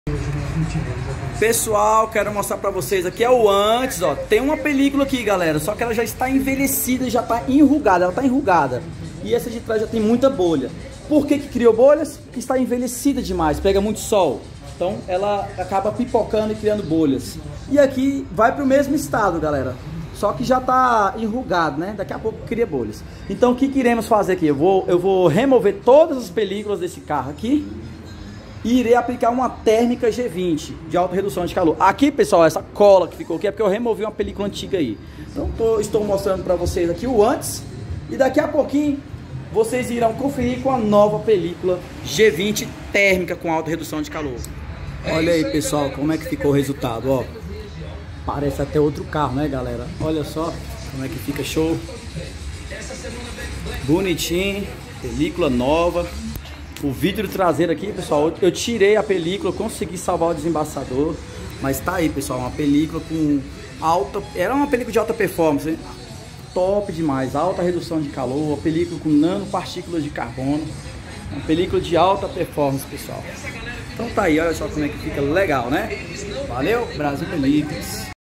Observem Como a Película Muda a Estética do Carro (G20 Termica)
Removemos a película antiga
(envelhecida com bolhas)

Aplicamos a Térmica G20
(alta redução de calor)

Bloqueio de raios UV
Redução de raios infravermelho

Melhor Visibilidade Interna
(fica clarinha de dentro para fora)

St. Placa das Mercedes - Núcleo Bandeirante, Brasília - DF, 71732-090